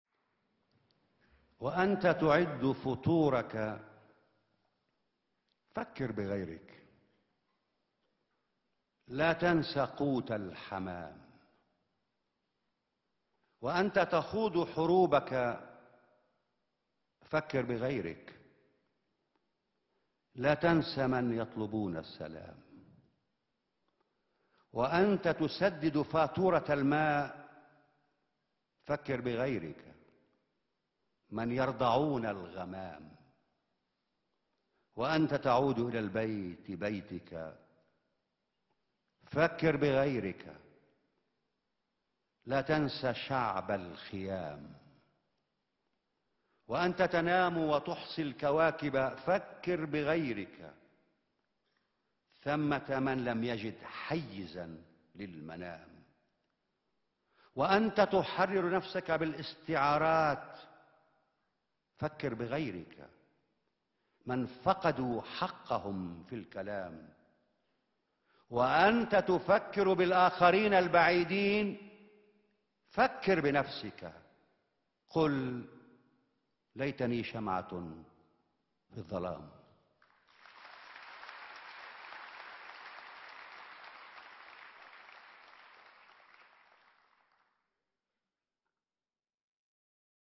Mahmoud Darwish: Think of Others
As you prepare your breakfast, think of others
(do not forget the pigeon’s food).
As you conduct your wars, think of others
(do not forget those who seek peace).
As you pay your water bill, think of others
(those who are nursed by clouds).
As you return home, to your home, think of others
(do not forget the people of the camps).
As you sleep and count the stars, think of others
(those who have nowhere to sleep).
As you liberate yourself in metaphor, think of others
(those who have lost the right to speak).
As you think of others far away, think of yourself
(say: “If only I were a candle in the dark”).